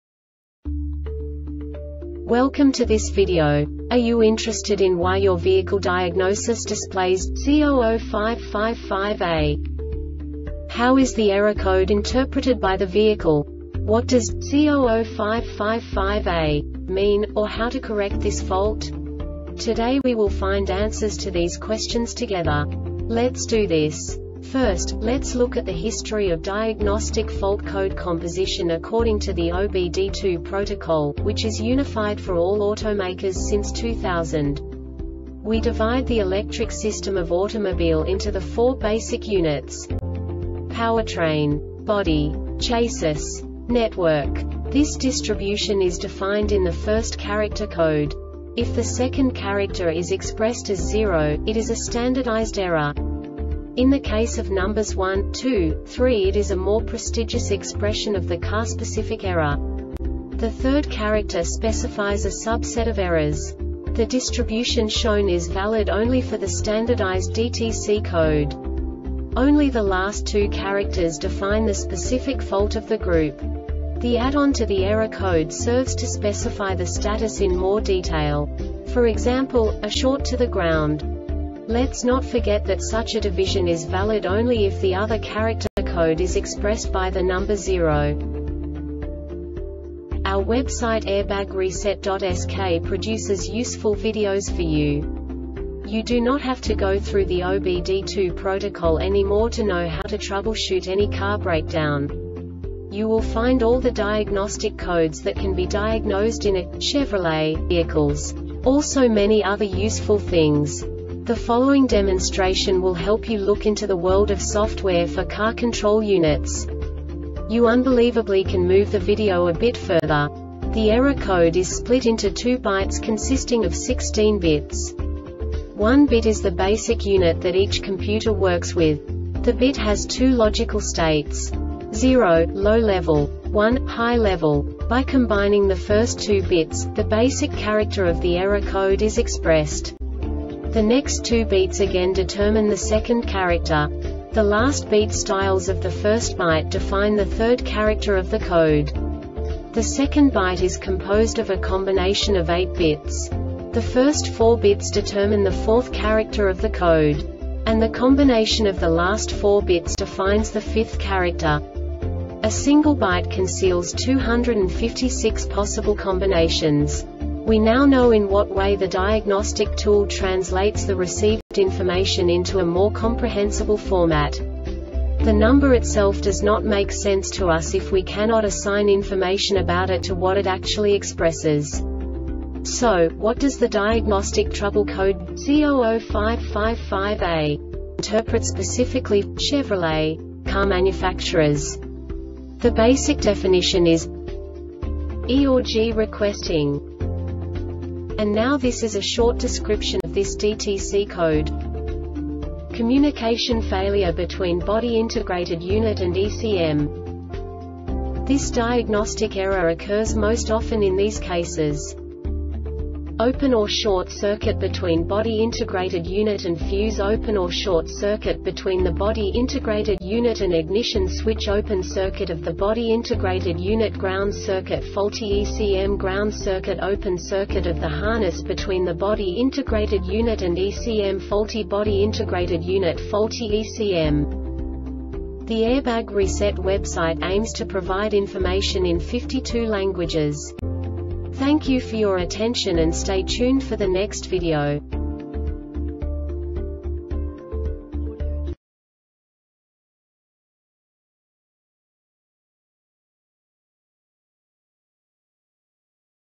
DTC Chevrolet C0055-5A Short Explanation
Contents:
0:21 Basic DTC analysis according to OBD2 protocol standard.
1:48 Insight into programming
2:58 A diagnostically understandable description of the Diagnostic Trouble Code
3:05 Basic error definition

Make:
Chevrolet
Code:
C0055 5A
Definition:
E or G request NG
Description:
Communication failure between body integrated unit and ECM.